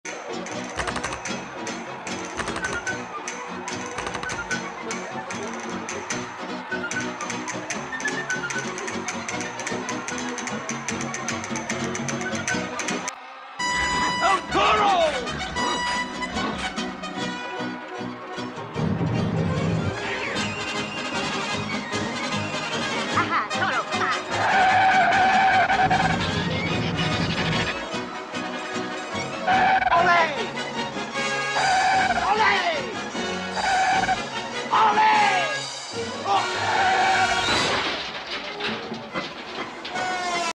Omanyala takes Kenyan relay 4*100m to the gold level
History as omanyala anchors Kenya to African relay title congratulations 👏🎉 Omanyala